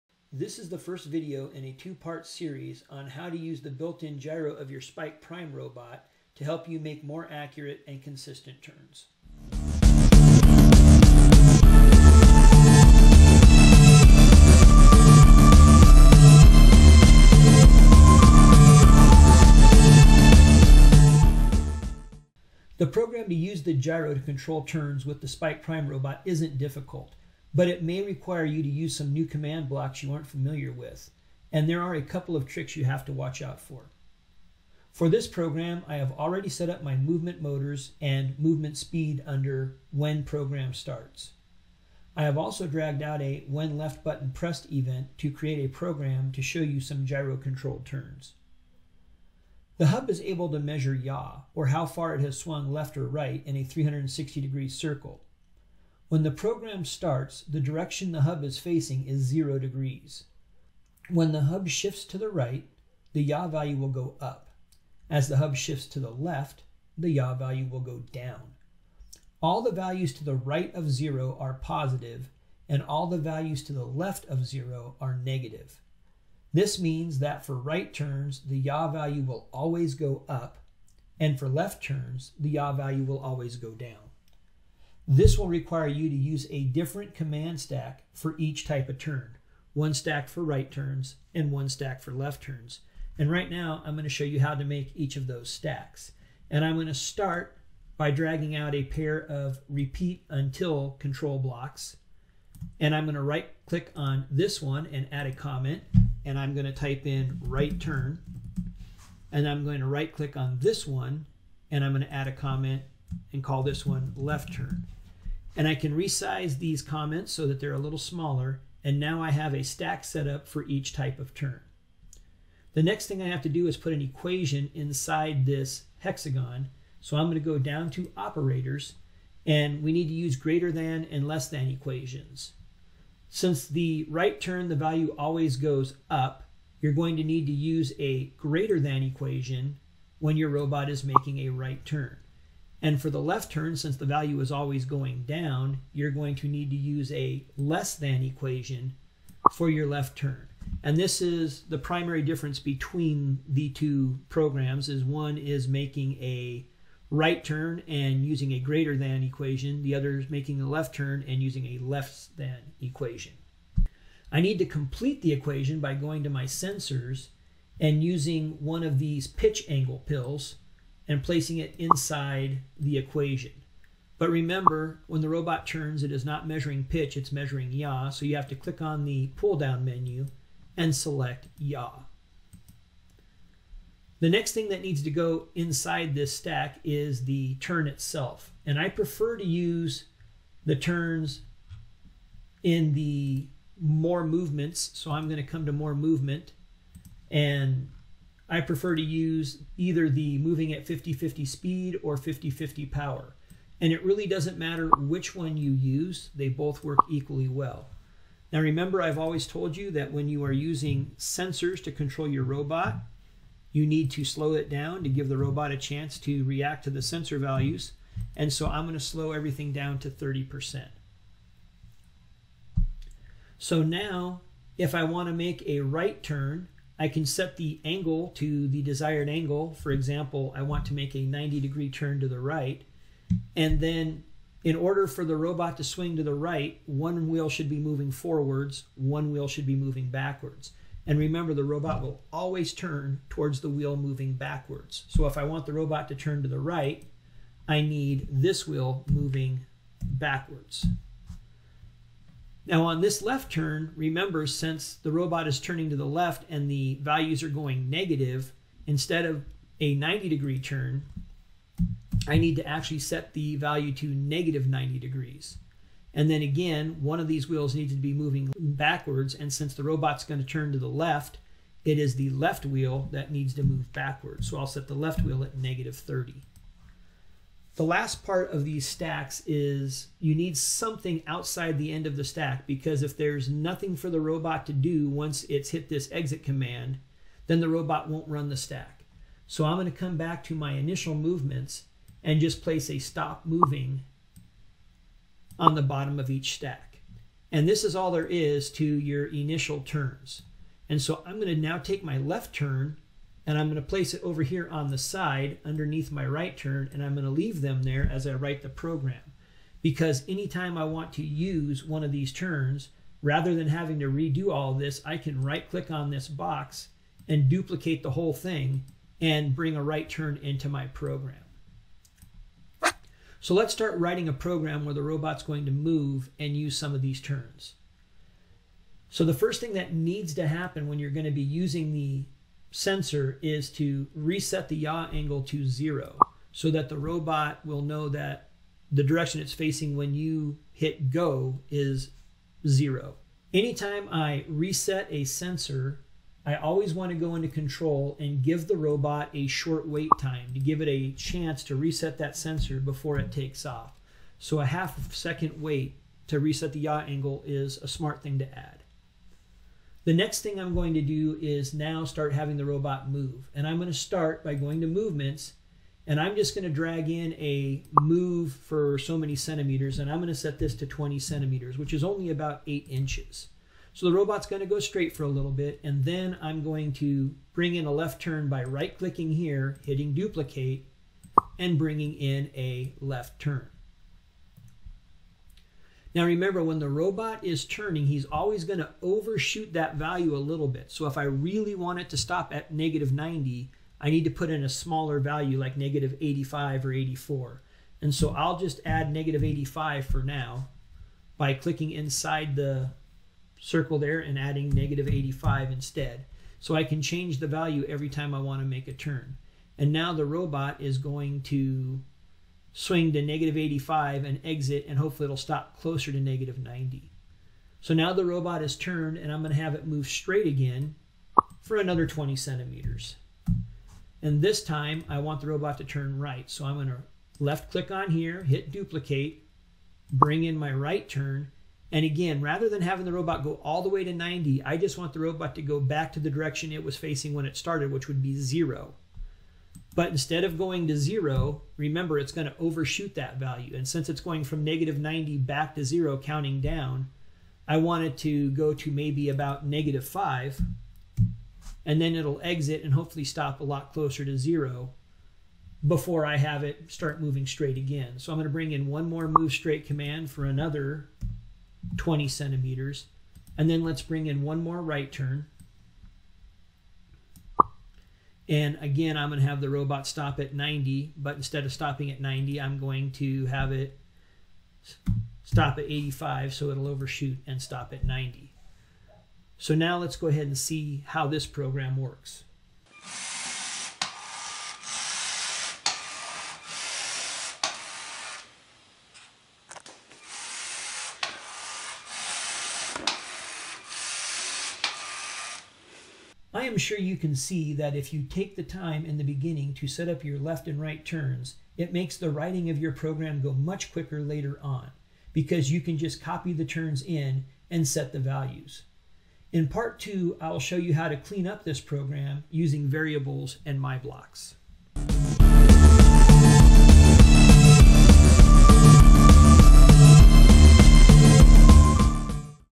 Spike Prime Gyro Controlled Turns Part 1
In this video, I will show you how to use the built in gyro on the Spike Prime or Robot Inventor robot to make more consistent and accurate turns.